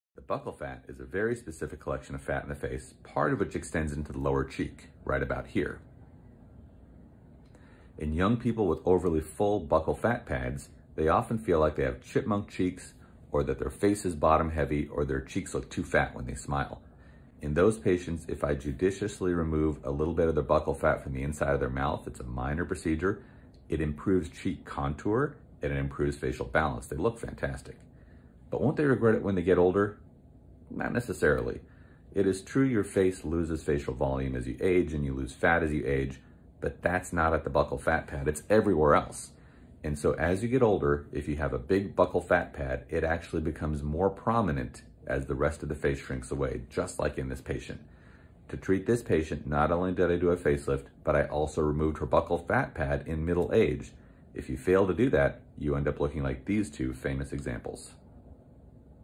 The Truth About Buccal Fat Reduction
The Truth about Buccal Fat Pad Reduction

Sure it looks great when you’re young, but you’ll regret it when you’re older!

Or will you?

Buccal fat in younger people is often associated with "chipmunk cheeks." Removing it can give more contour to a face. But will people regret the operation as they get older. While it's true that facial volume decreases as we age, this isn't the case with buccal fat.